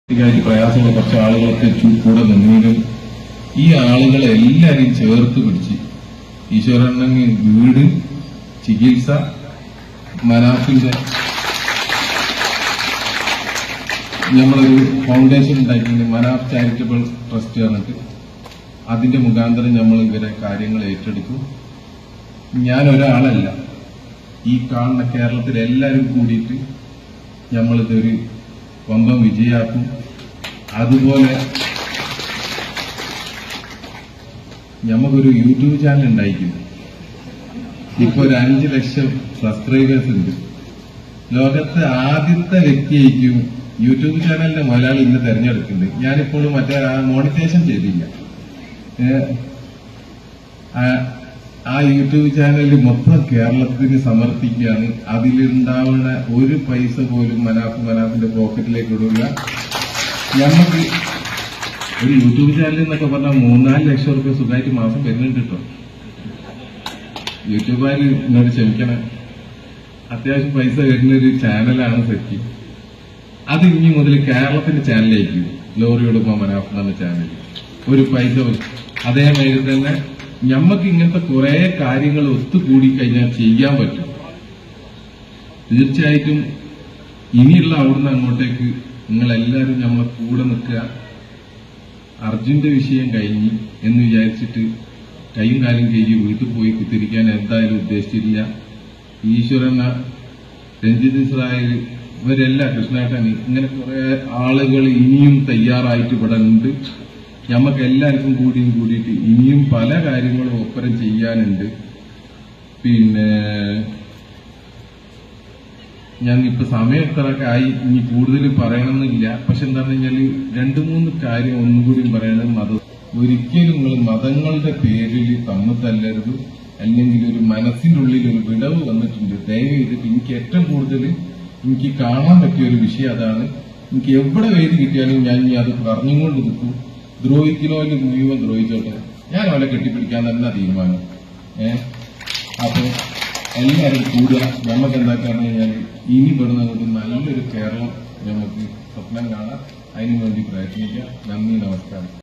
മല്‍പേയും കുടുംബവും ഇനി മനാഫിന്റെ കൈകളില്‍ ഭദ്രം | Manaf & Malpe Real love story from Kerala
മല്‍പേയും കുടുംബവും ഇനി മനാഫിന്റെ കൈകളില്‍ ഭദ്രം, കുഞ്ഞുങ്ങളുടെ ചികത്സ ഏറ്റെടുക്കും, വീട് നിര്‍മ്മിച്ച് നല്‍കും